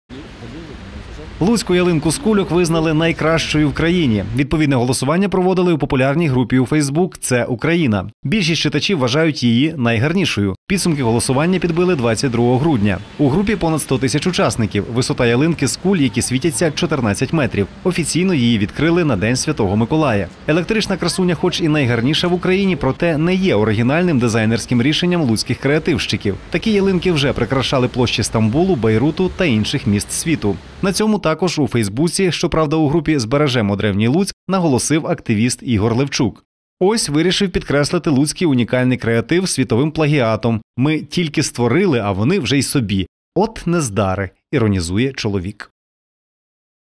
Луцьку ялинку визнали найкращою
Луцьку ялинку з кульок визнали кращою в країні. Відповідне голосування проводили у популярній групі у Facebook "Це Україна". Більшість читачів вважають її найгарнішою. Підсумки голосування підбили 22 грудня. У групі понад 100 тисяч учасників. Висота ялинки з куль, які світяться, − 14 метрів. Офіційно відкрили її на день святого Миколая.

Електрична красуня хоч і найгарніша в Україні, проте не є оригінальним дизайнерським рішенням луцьких креативщиків. Такі ялинки вже прикрашали площі Стамбулу, Бейруту та інших міст світу.
На цьому також в ФБ, щоправда у групі "Збережемо древній Луцьк" наголосив активіст Ігор Левчук.

"Ось вирішив підкреслити луцький "унікальний" креатив світовим "плагіатом"... "Ми" тільки створили, а вони вже й собі... От нездари...", − іронізує чоловік.